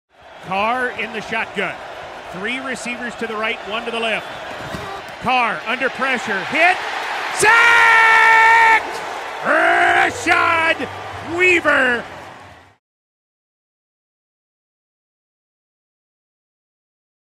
Rashad Weaver Sack Against Raiders | In the Booth
Mike Keith calls OLB Rashad Weaver touchdown against the Las Vegas Raiders during Week 3 at Nissan Stadium.